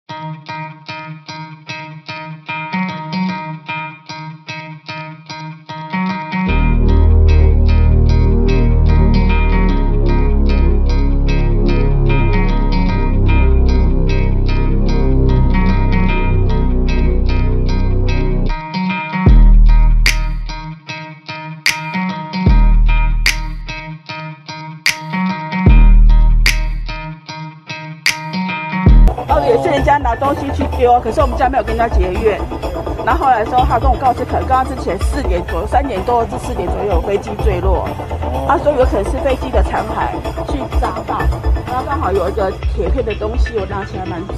【井中莱蛙061】绿蛙今日损失一架反潜直升机！台军方一架S70C在高雄基地坠毁，坠机零件砸中民宅，住户称“以为被寻仇”。
绿蛙今日损失一架反潜直升机！台军方一架S70C在高雄基地坠毁，坠机零件砸中民宅，住户称“以为被寻仇”。